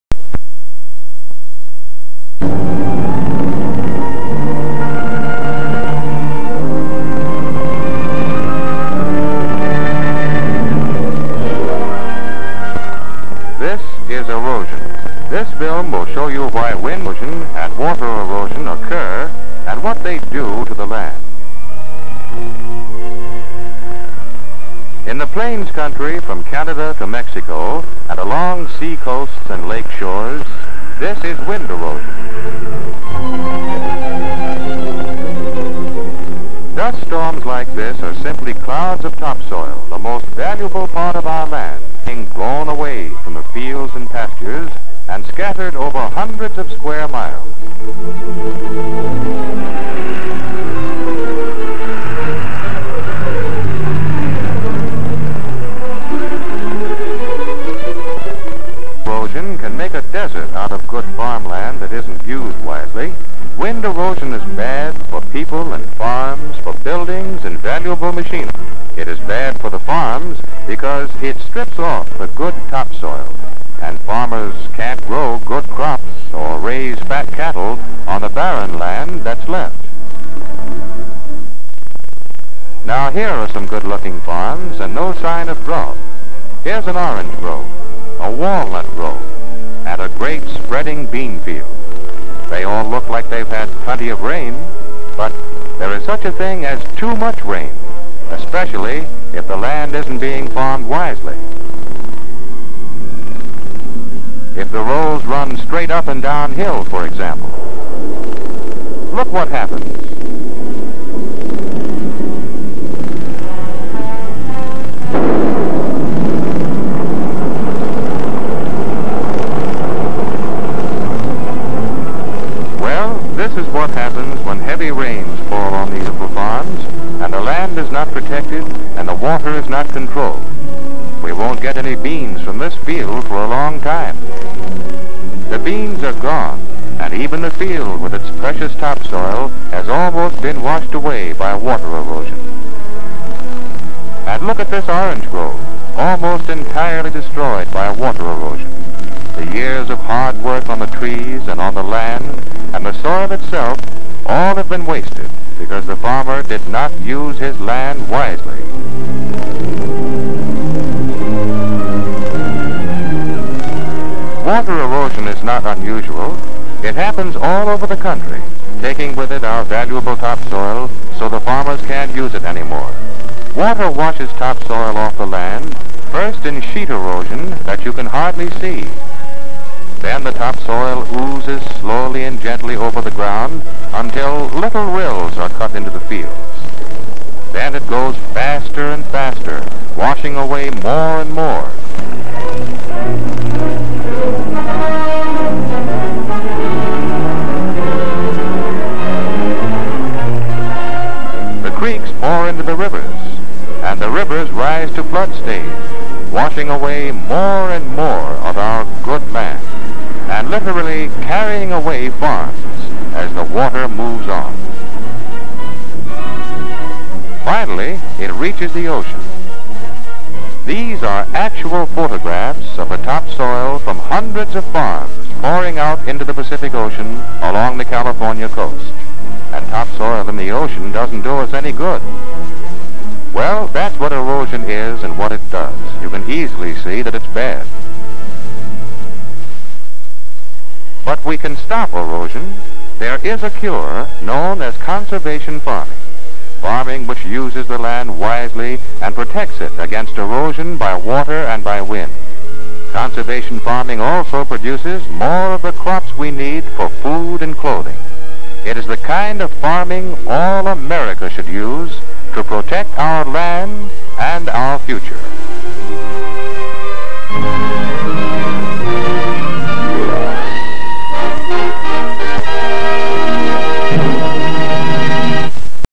Erosion
Reveals the common causes of erosion--water, wind, ice and roots. Shows how our land is constantly being changed.